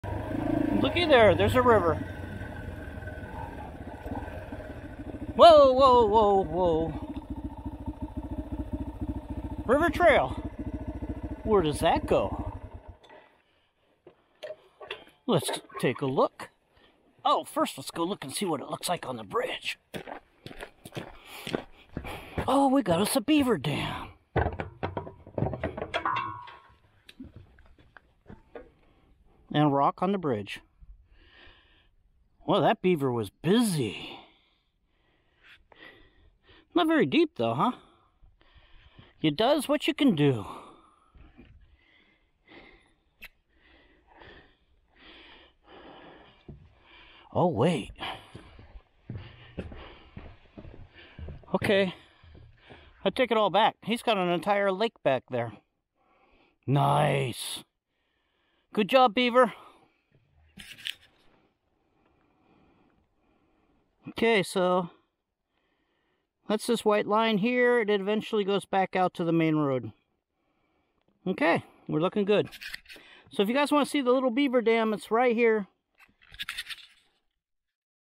Tahuya beaver dam sighted
Wow, stopping to see the sights pays off. I found a small beaver dam with an entire lake behind it.

Tahuya river trail, Washington State, June 25, 2024